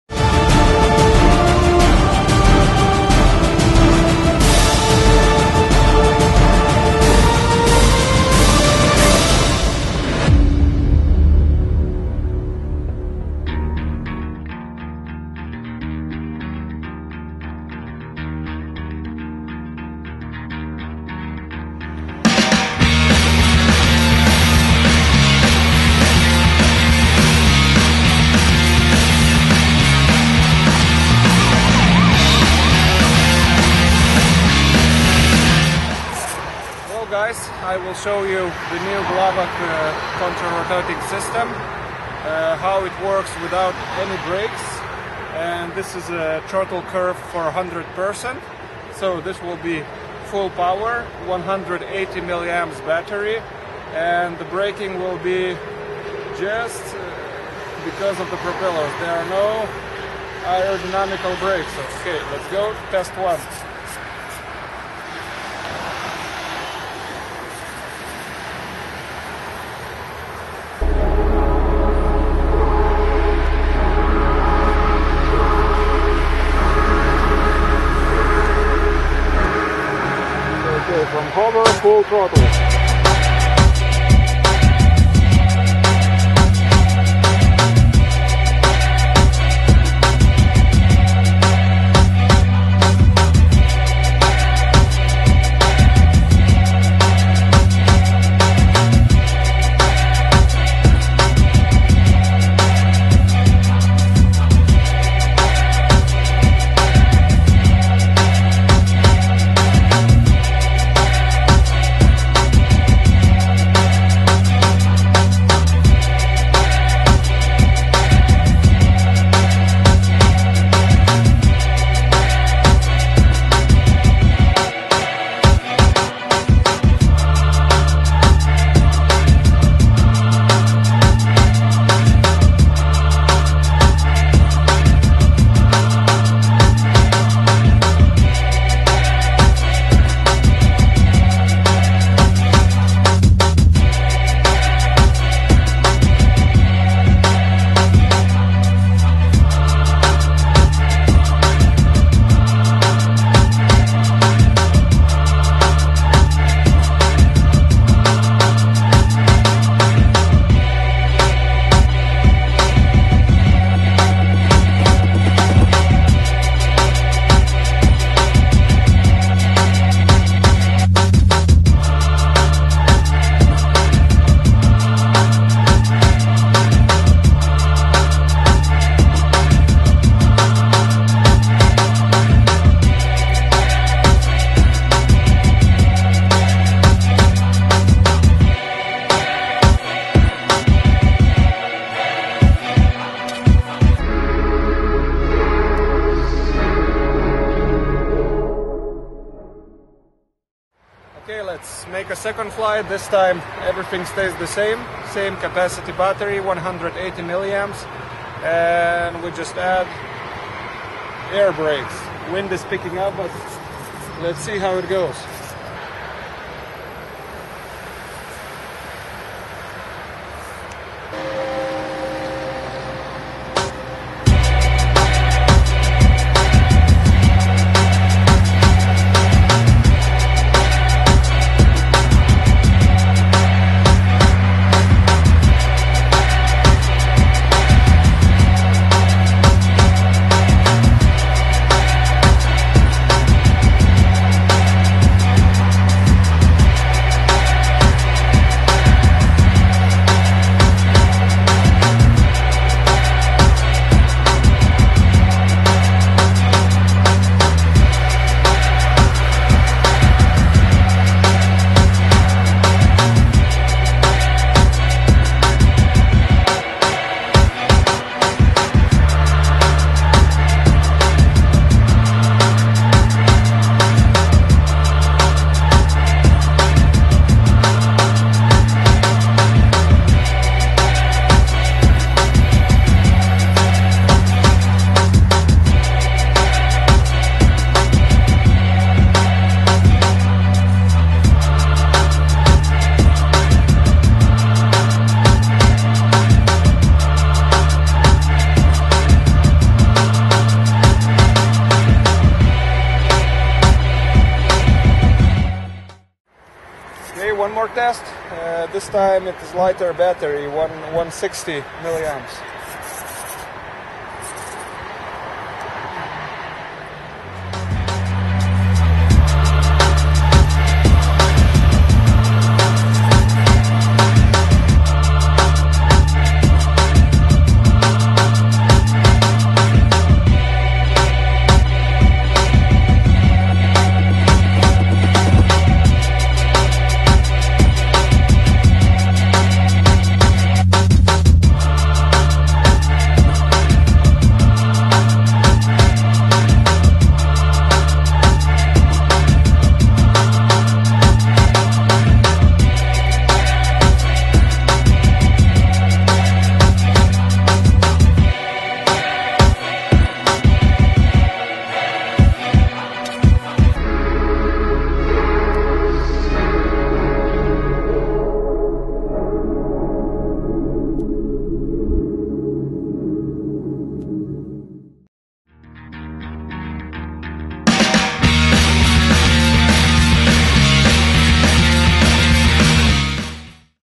New Coaxial system tested on "Anubis".
Goal of this video is to demonstrate unique coaxial engine developed for F3P (indoor aerobatic airplanes). System is  developed in Germany by Mr. Sergej & Alex Glavak. Single bruschless motor require just one ESC and it turn (opposite direction) two 10 inch V shape carbon propellers. More information about this system: pauzuolis-rc.com